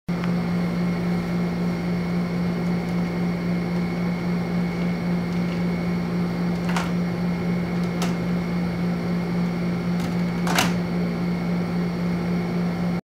LEGO Clock Tower Mechanics Prototype
I wanted to be able to control all of the mechanics of the clock tower with a single lever/ knob in the back. It took me awhile to figure out how to get the entire thing to reset when you pull the lever down but I finally got the results I want. The only thing I was unable to get working is for the platform to tip back when you raise it. It requires a small manual tap. That would be something to fix if I go further with this project.